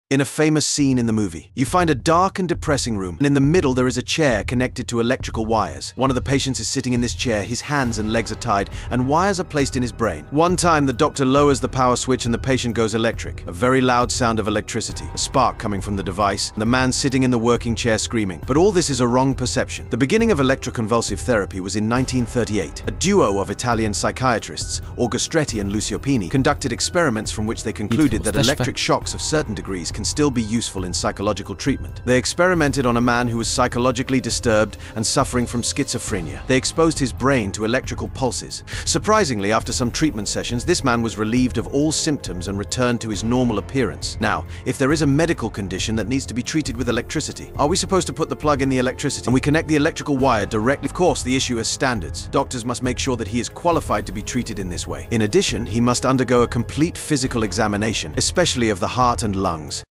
Is electroshock therapy still used in psychiatry?😱👀😲🔥
Modern ECT is a safe procedure, which is used to treat the most severe forms of depression (including psychotic depression) and severe manic symptoms, when the situation is thought to be life-threatening or after all other treatment options have failed.